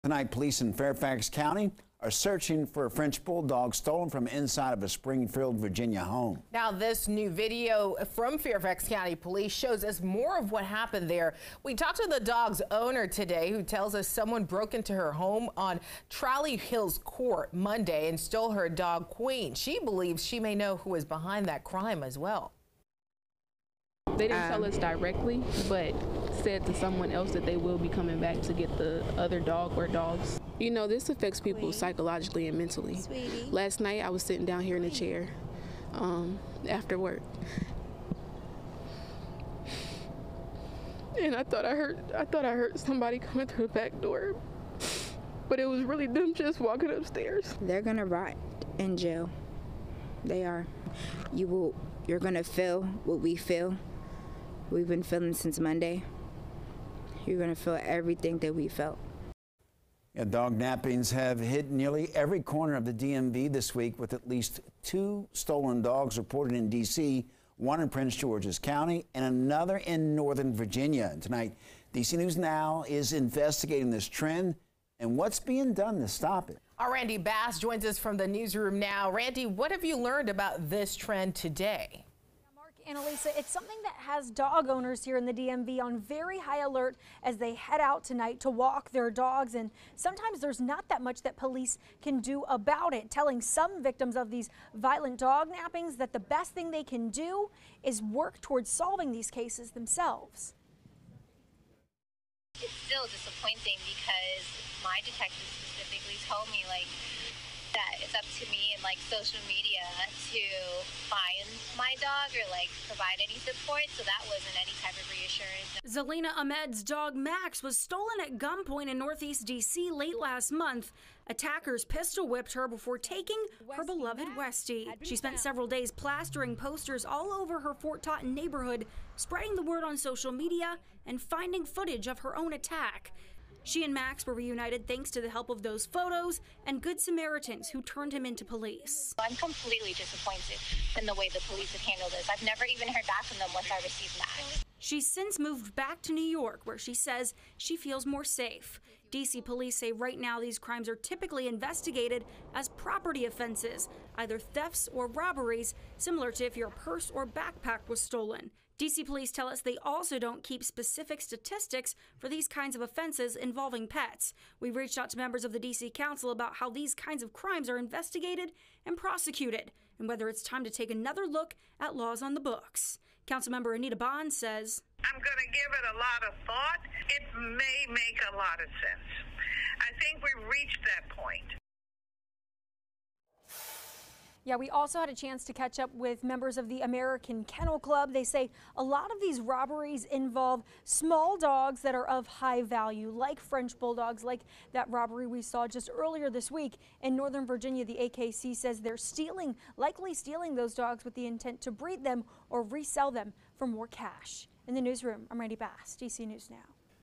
Burglar steals French bulldog from Springfield home
The Fairfax County Police Department said someone burglarized a Springfield home and tried to take all three dogs that lived in the home.